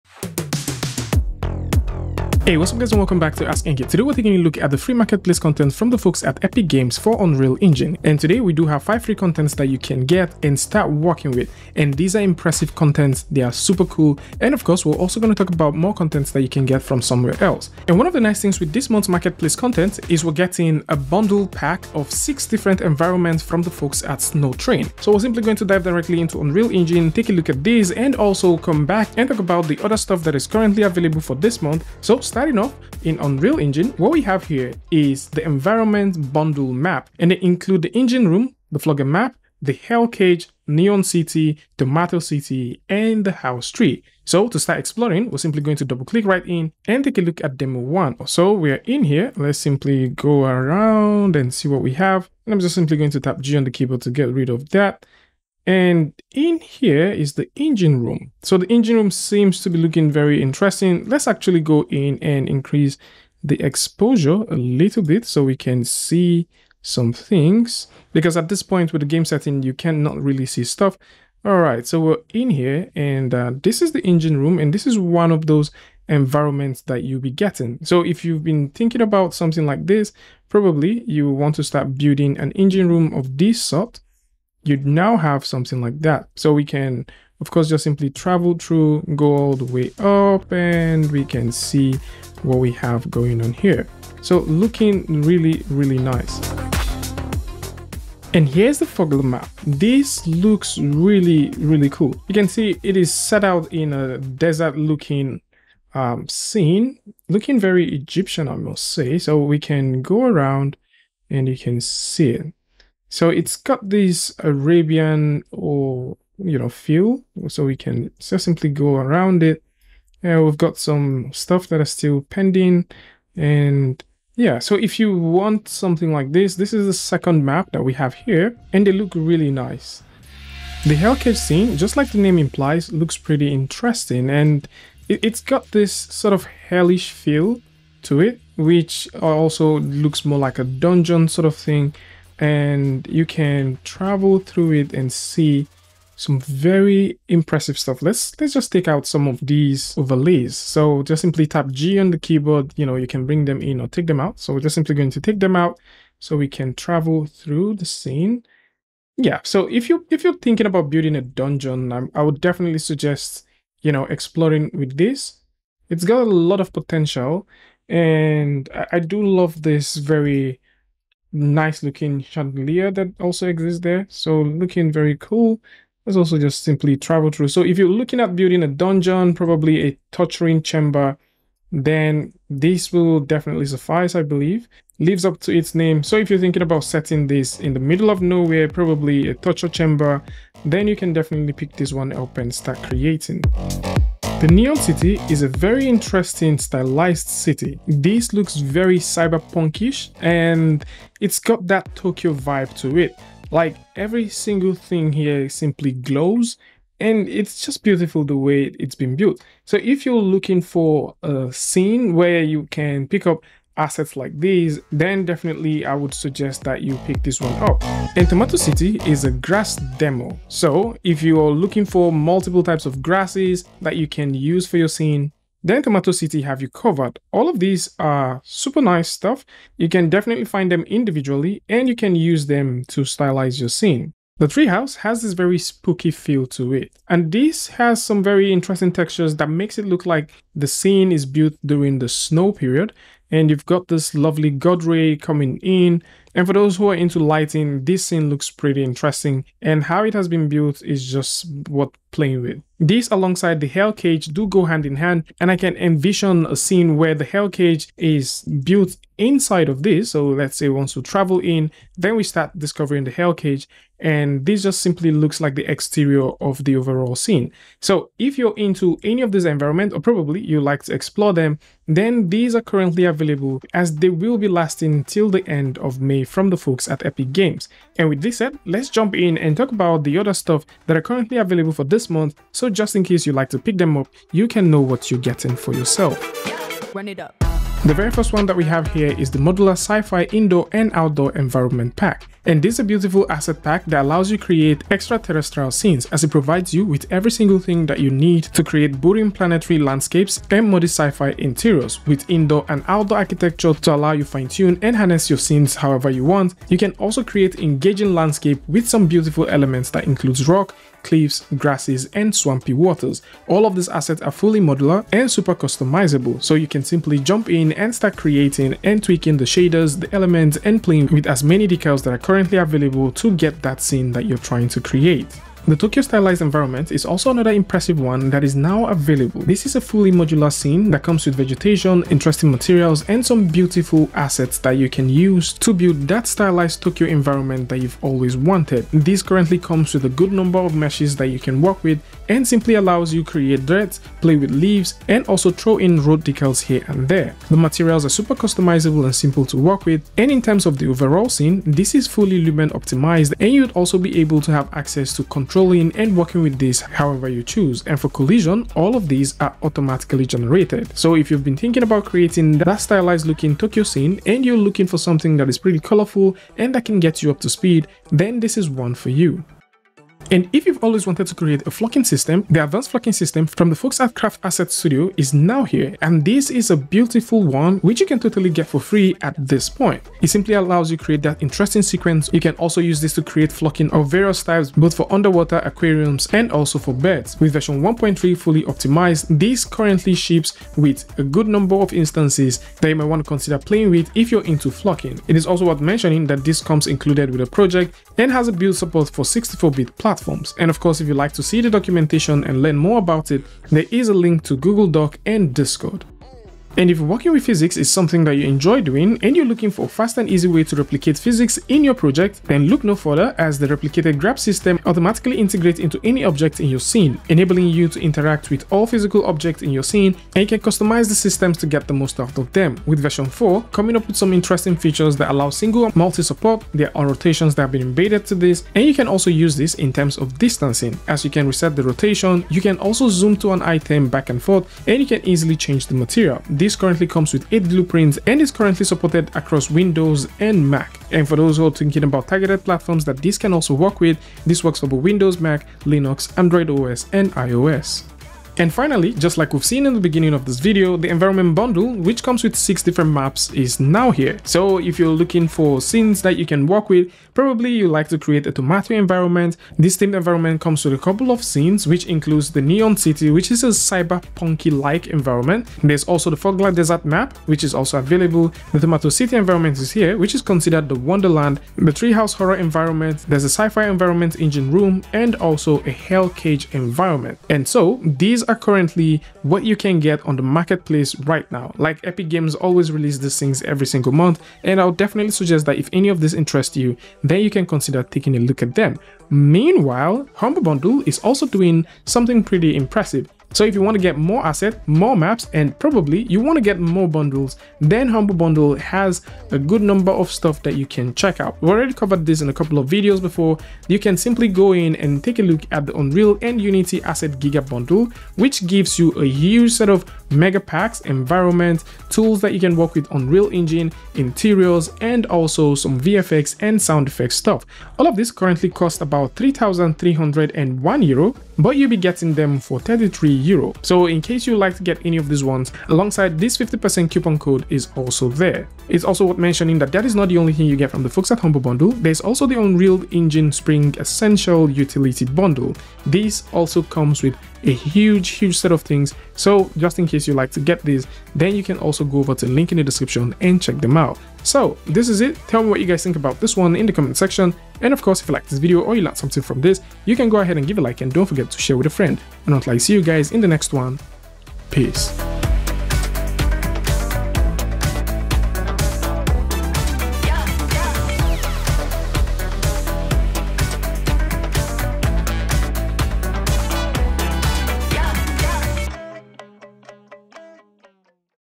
Free Unreal Engine Assets For May 2024!
Your Monthly free assets are here.

00:00 Intro
00:26 Exploring the Multi Bundle Pack.
00:57 Engine Room
01:38 Forgler Map
02:10 Hell Cage
03:14 Unreal Neon City [Cyberpunk]
03:38 Unreal Tomato City
03:54 Tree House [Spooky]
04:44 The Other Stuff
04:49 Modular Sci-Fi Indoor/Outdoor environment.
05:25 Tokyo Stylized Environment
06:12 Advanced Flock System
06:47 Replicated Grab System
07:31 Environment Bundle
08:09 More Assets & Environments
09:09 Final Words